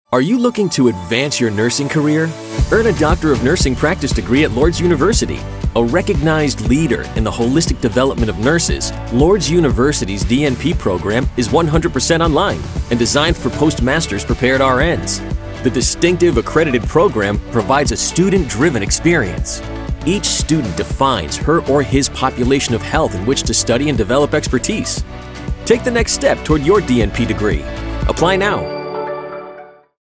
Lourdes University Doctor of Nursing Practice Program
An online DNP program designed for post master's prepared RNs.